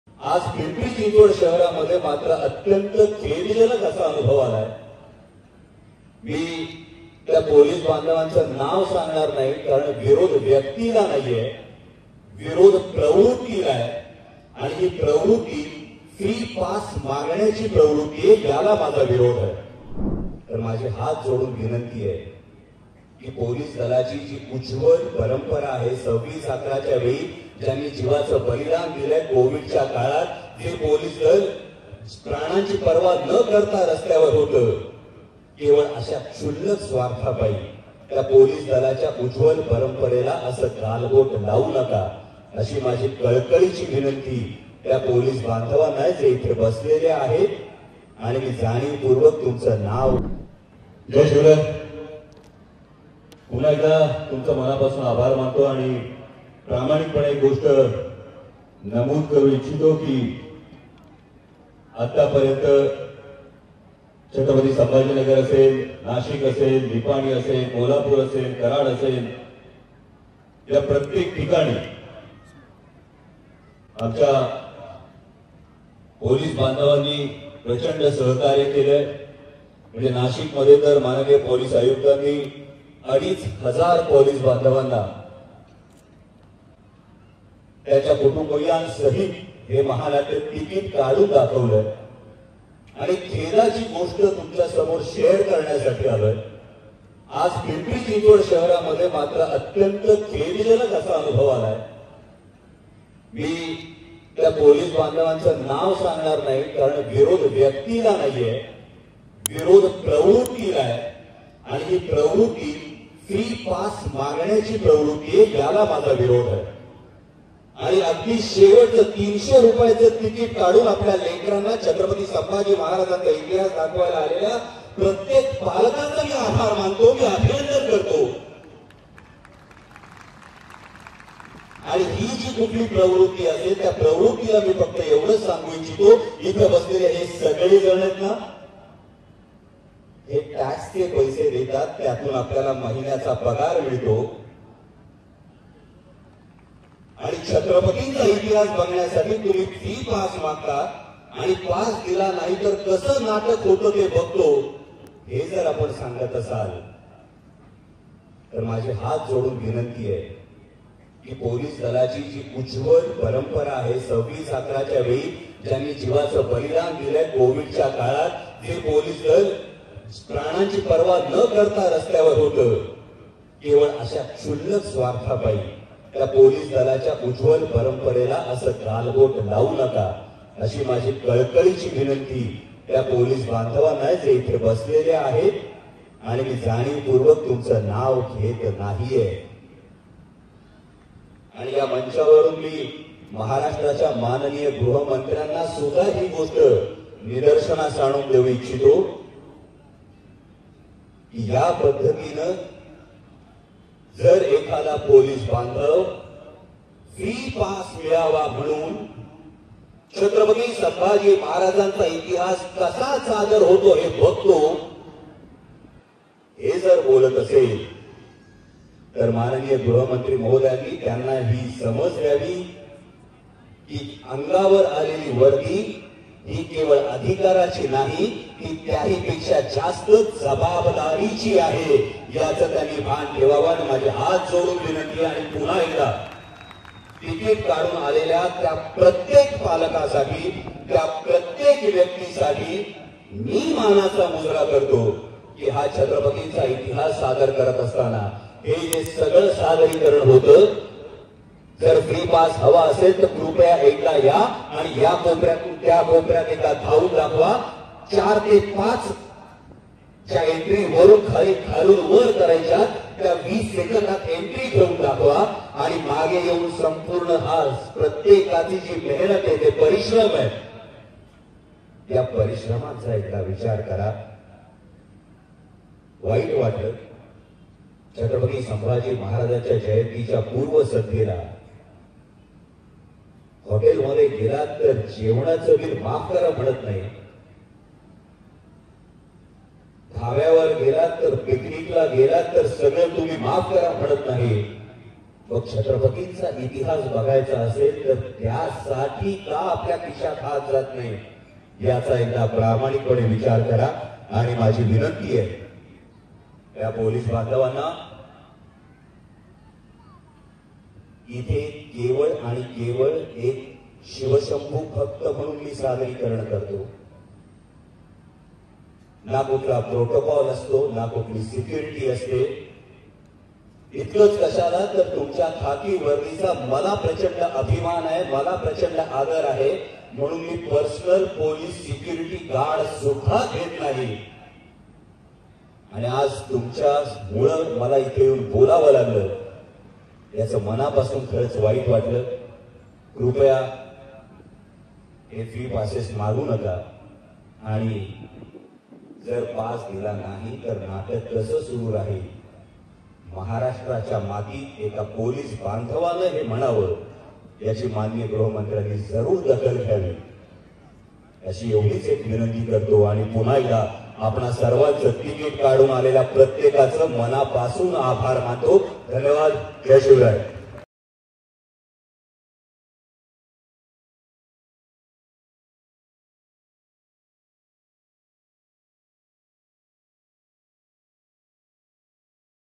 Amol Kolhe | फ्री पास मागणाऱ्या पोलिसांना अमोल कोल्हेंनी सुनावलं | LetsUpp Marathi
पुण्यातील पिंपरी चिंचवड येथे शिवपूत्र संभाजी महानाट्य सादर करत असताना पोलिसांकडून फ्री पासची मागणी करण्यात आल्याचे खासदार अमोल कोल्हे यांनी आरोप केले आहेत. यावेळी अमोल कोल्हेंनी या पोलिसांना चांगलेच सुनावले आहे. पास मागणाऱ्या पोलिसांना कलाकार म्हणून विनंतीही केली आहे.